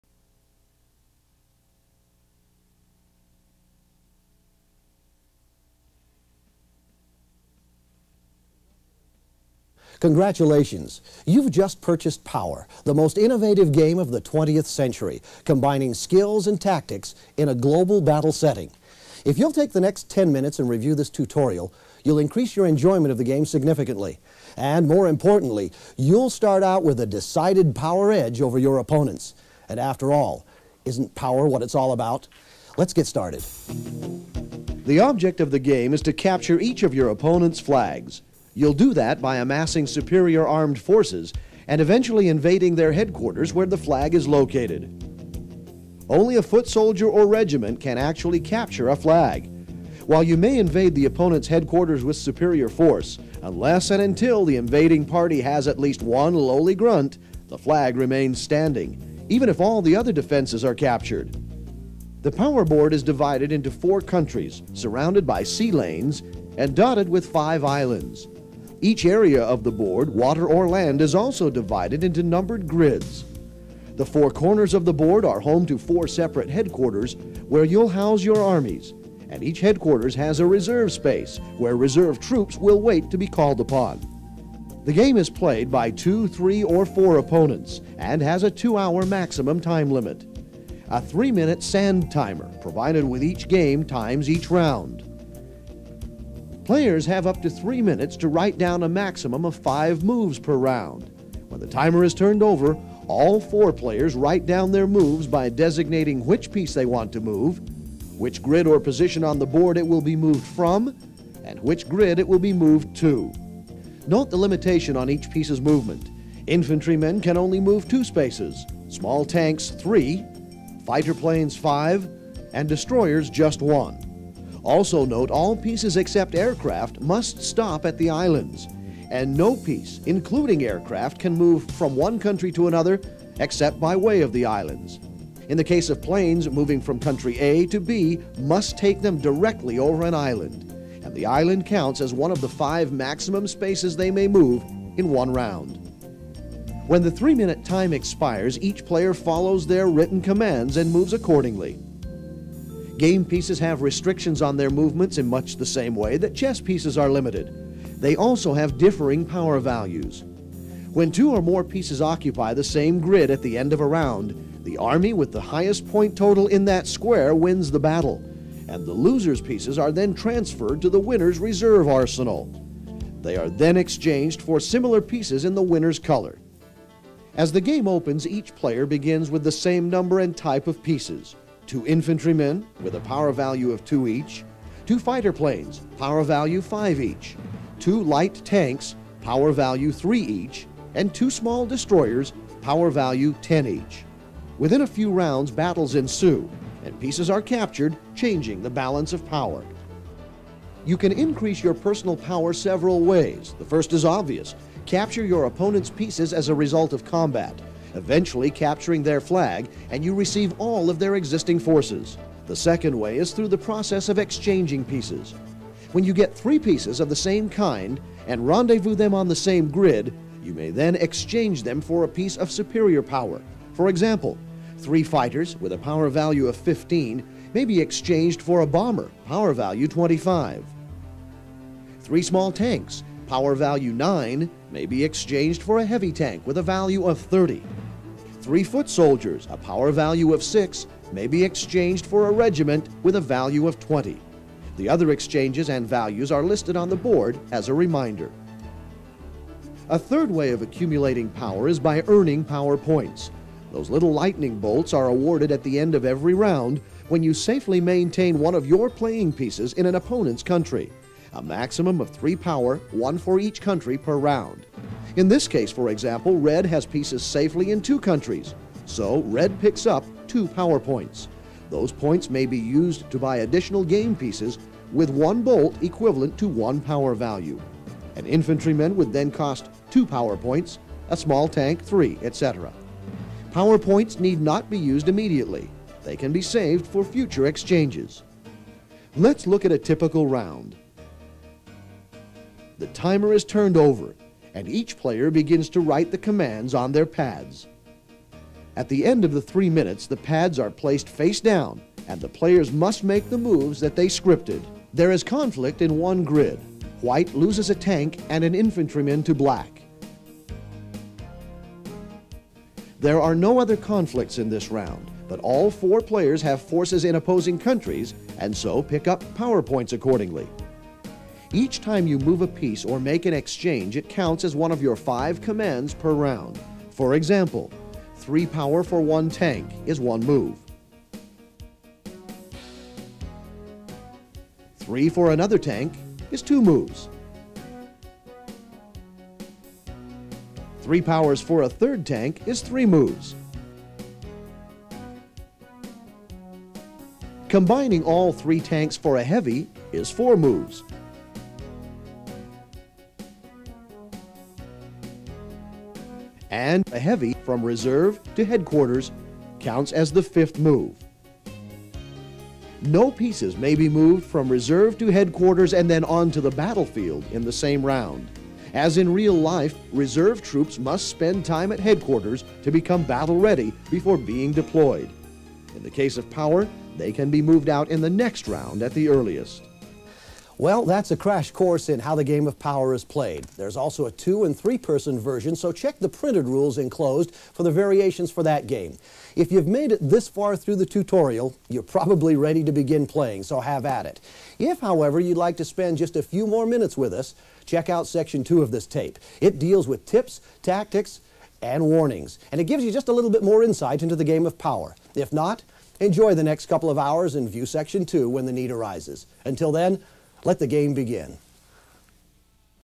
POWER: The Boardgame - 1/2: Tutorial
This is the first half of the VHS cassette bundled with the mid-90's edition of POWER: The Boardgame.